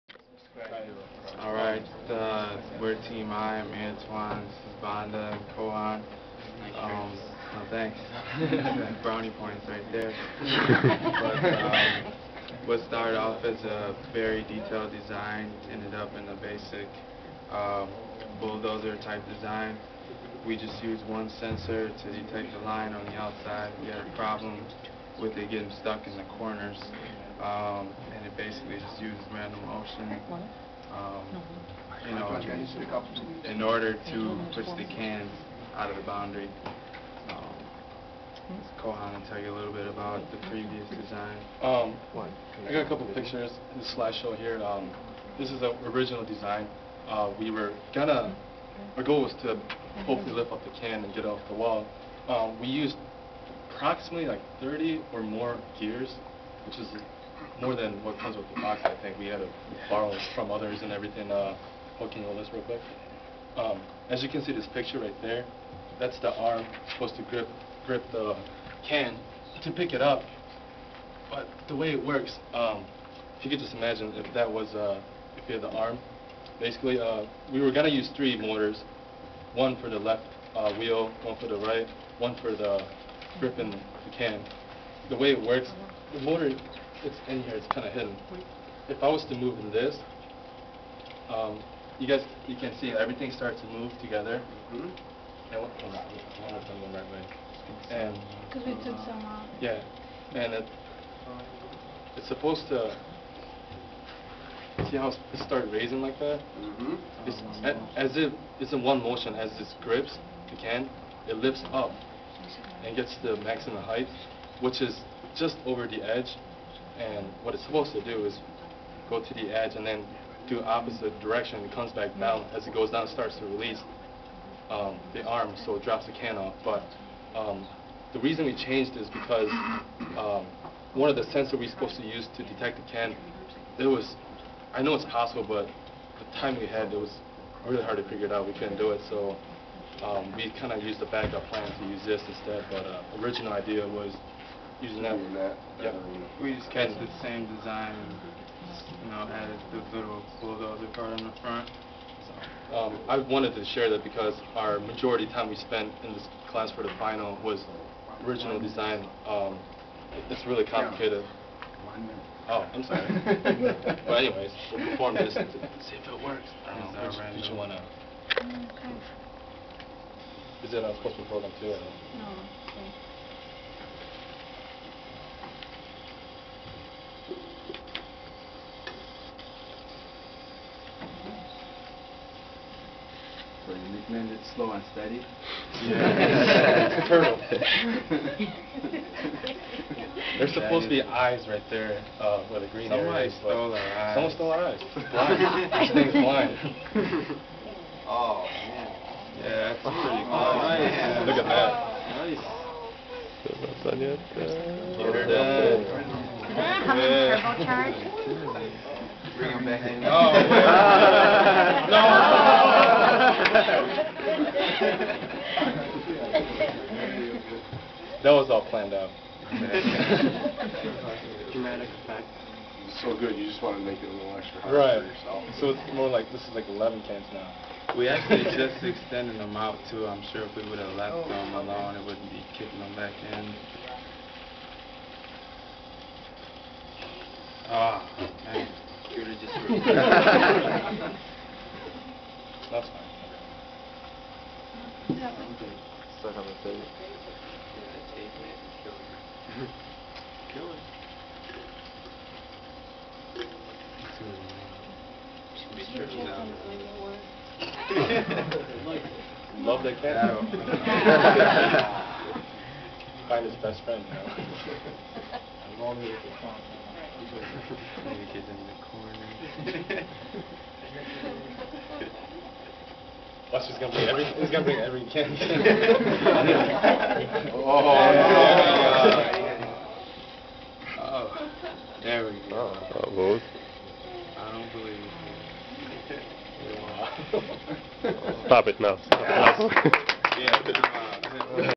junkbot
the lego robot needs to stay within the domanin and knock the cans out.